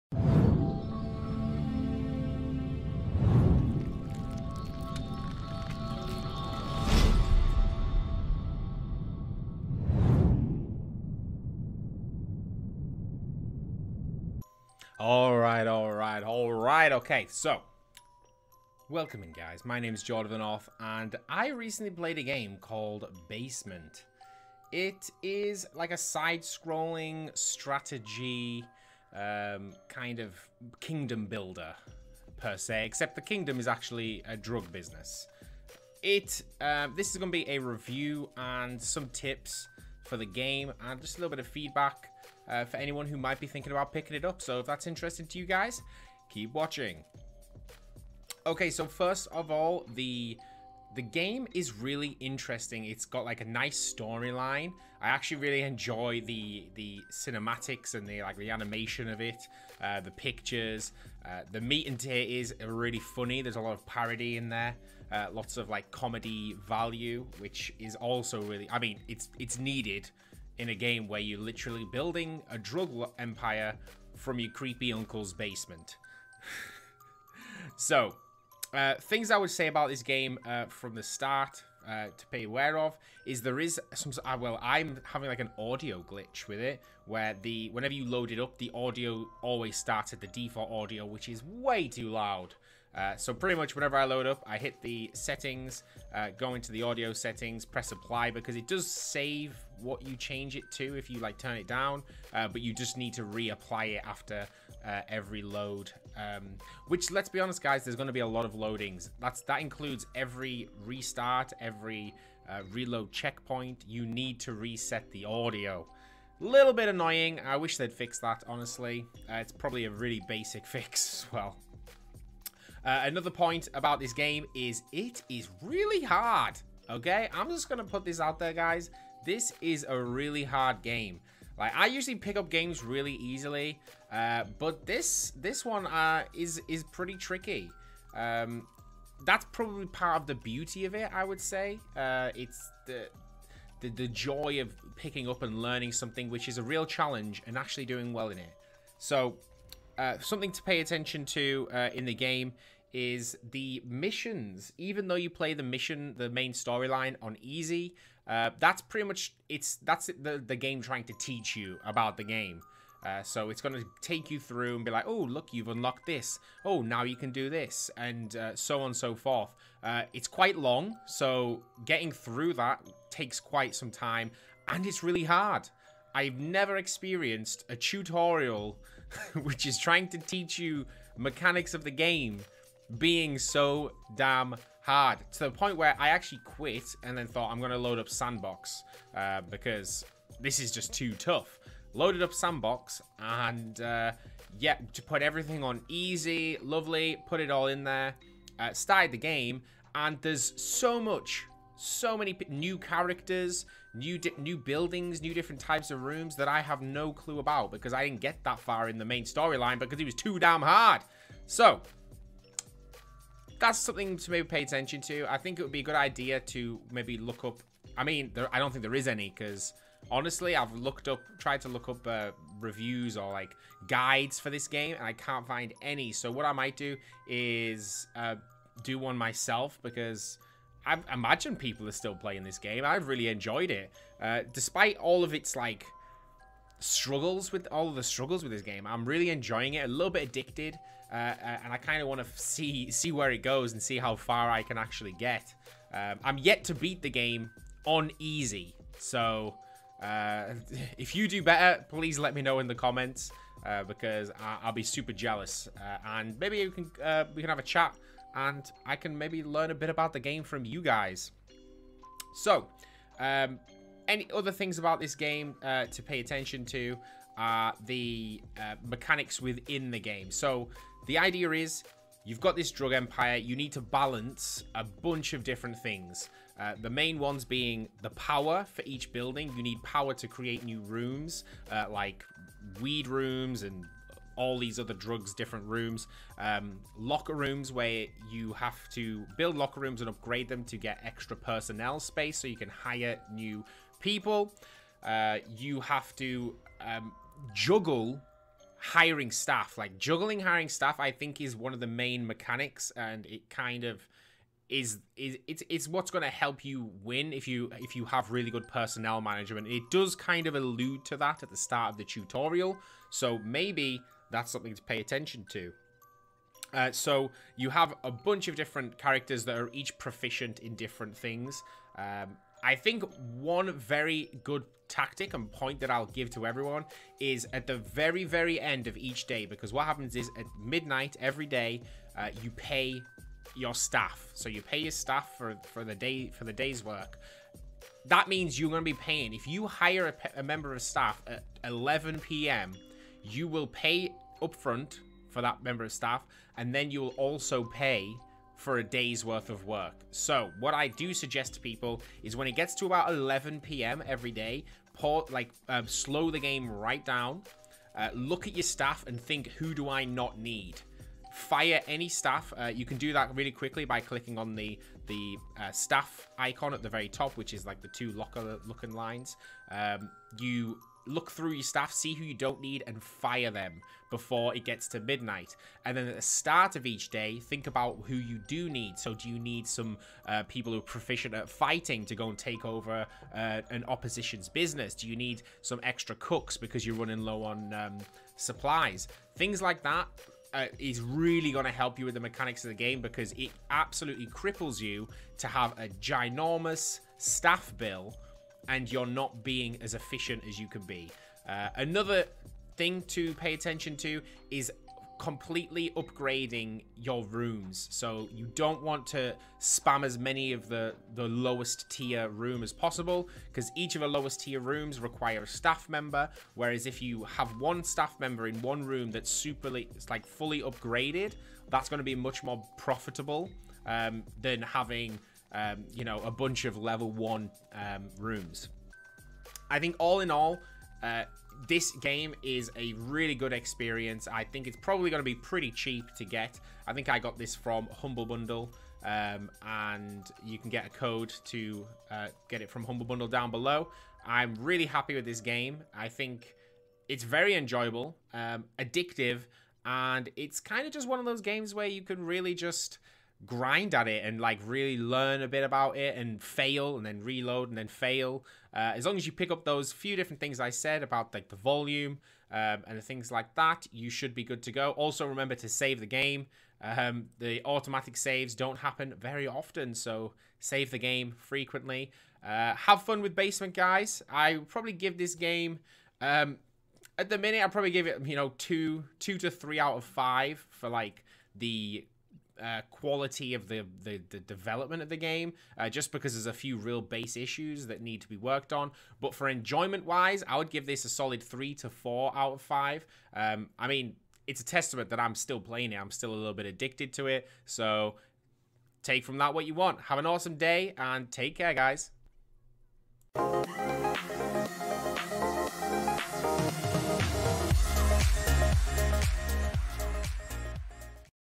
BASEMENT Game: Review & Tips for Beginners
I played Basement for a few hours, heres my thoughts and feedback.

Basement is a drug-dealing strategy/simulation game. You play as a great scientist that has been forced to do not exactly legal stuff underground, as nobody wants to support his idea. Basement is all about management.

Hit that Ding Dong bell to get Notified whenever I make a NEW UPLOAD

My schedule is on my channel